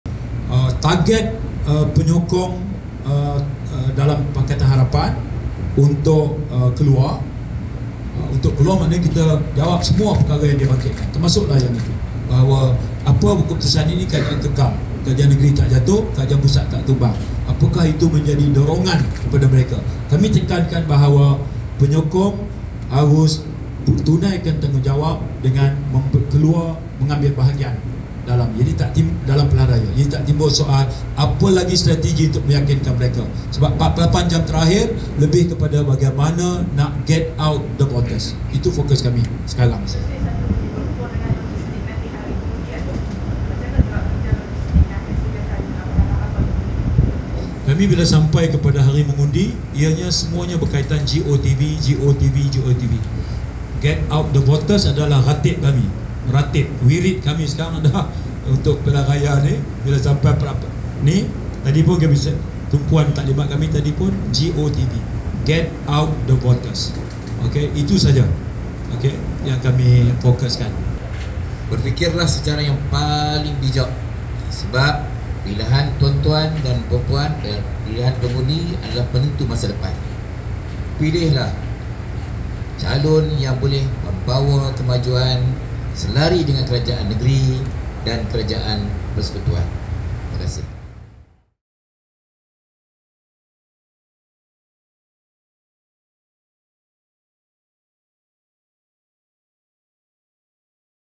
Saifuddin: Perikatan's 'green wave' has diminished to a ripple
The “green wave” of Perikatan Nasional has diminished to a ripple, said Pakatan Harapan secretary-general Datuk Seri Saifuddin Nasution Ismail in a press conference on Thursday (July 4).

He said Perikatan falsely used the term during last year's state election, telling voters that the opposition coalition could topple the unity government coalition.